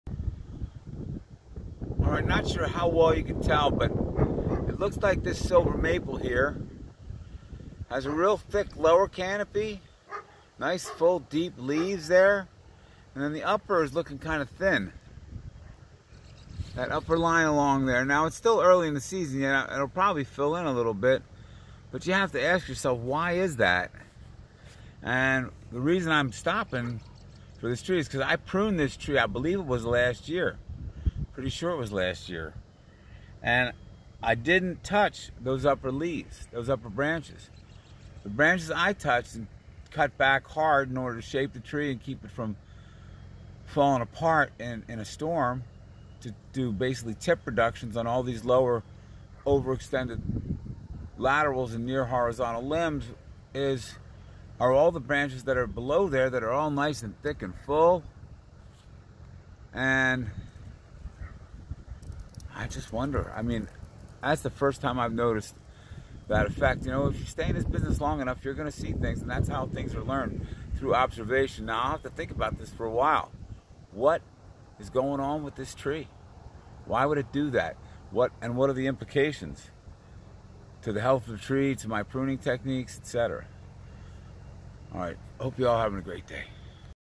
Strange Pruning Response from Silver Maple
After driving by this tree I pruned last year and showed on video, it seems very obvious that leaves on the pruned limbs, which are the entire lower and middle canopy, came in much thicker than the unpruned limbs in the upper canopy. It's still fairly early in the season and we've had an extraordinarily cold spring. ANd I have never observed anything like this effect before. Does anyone have any idea what might be going on?